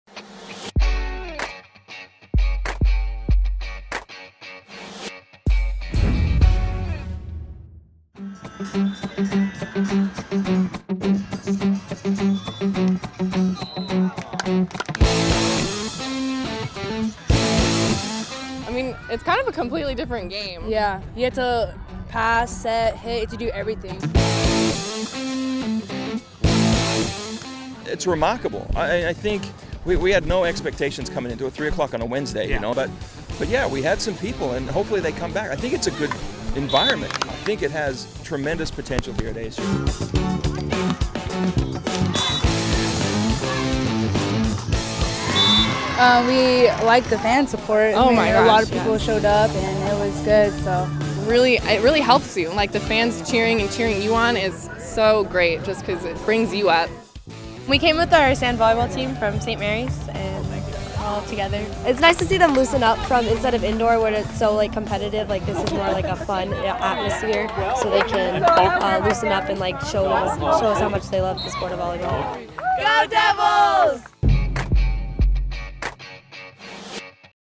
Sights and sounds from Sun Devil sand volleyball
(Originally published on 04/02/2014) Athletes and fans talk about the fun atmosphere so far during the first season of ASU sand volleyball. (Courtesy of TheSunDevils.com)